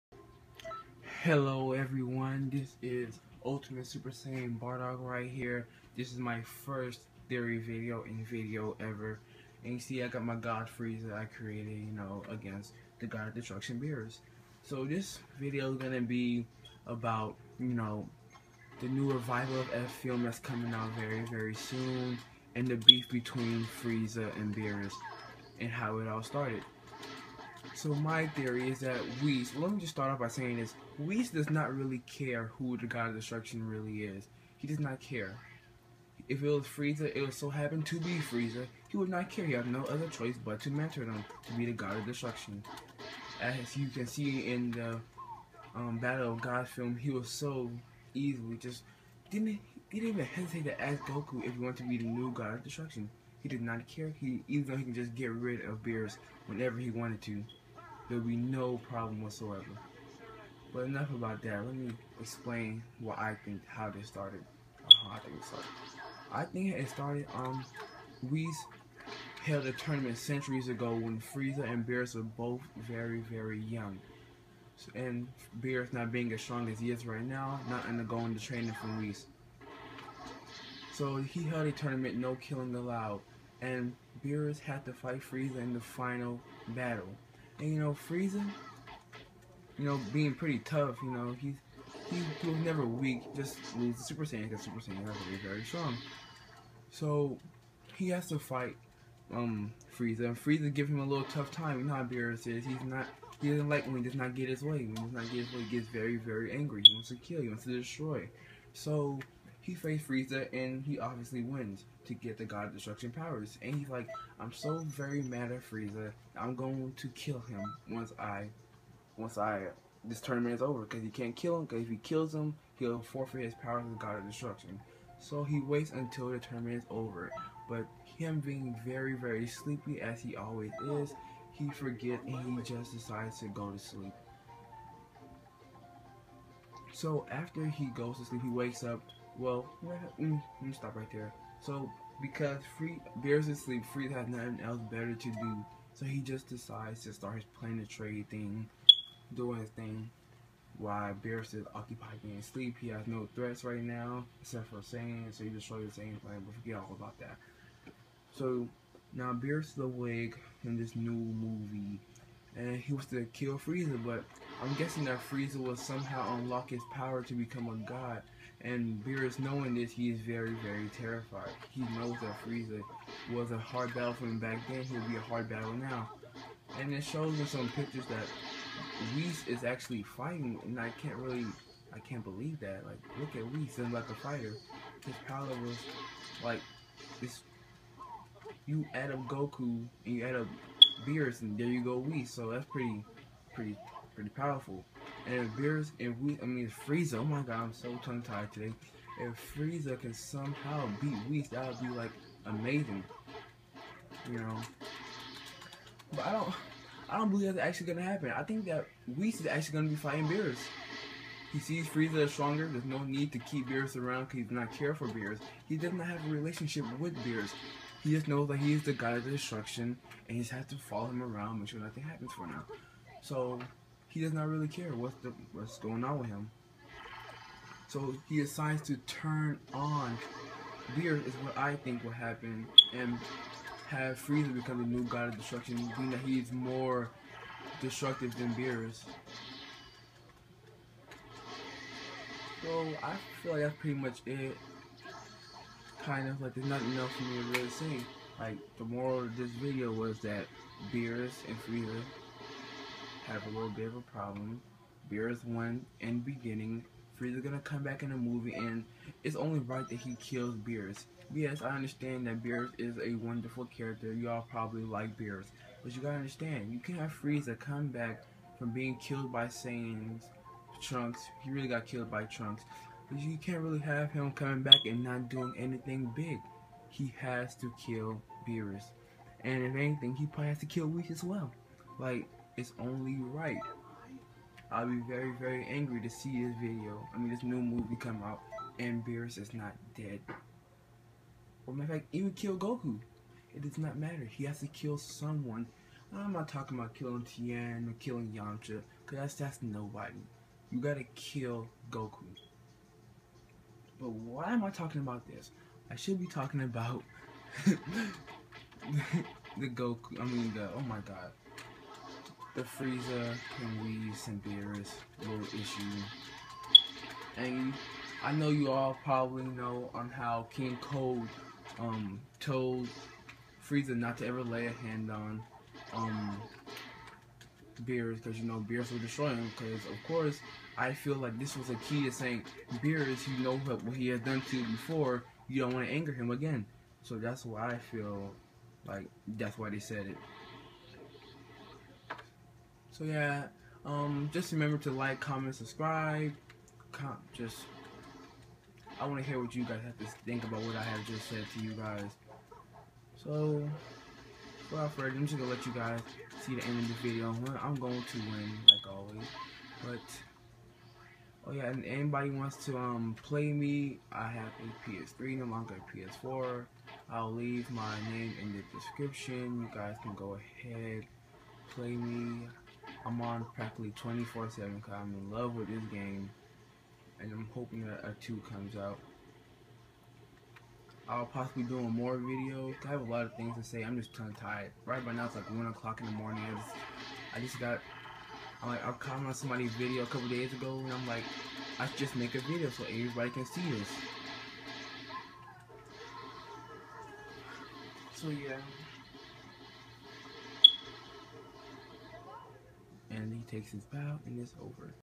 How did Frieza and Beerus rivalry start Theory
PSN: JsmooVe4587.                                                                  In this video I'm going to be talking about the frieza and Beerus and how their rivalry most likely began and a little about the Resurrection of  F fiml.